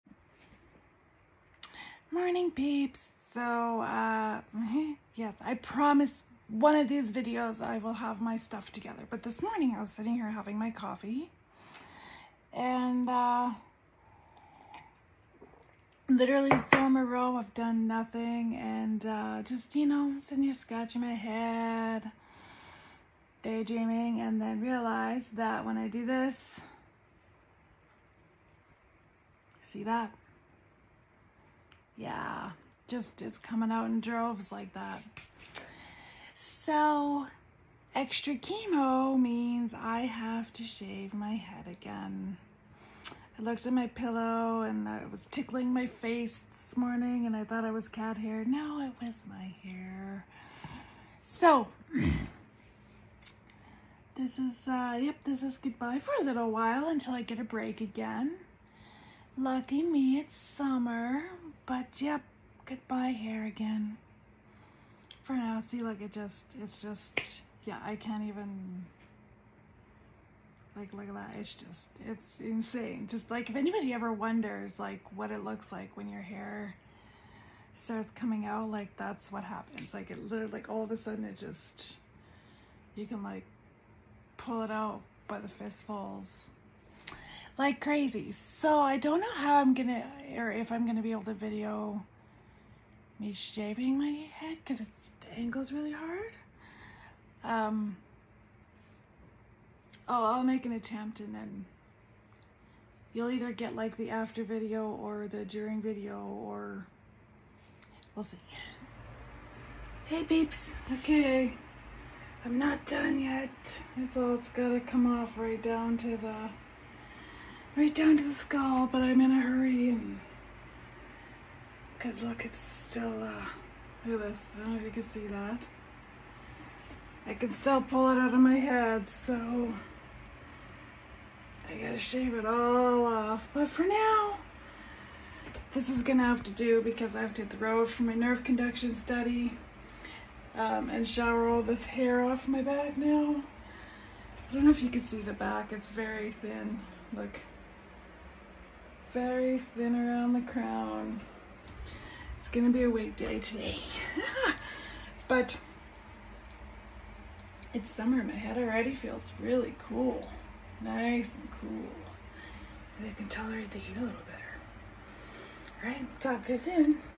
Hair Say Bye
With extra chemo so goes the hair again but that just means cancer getting Butt kicked again.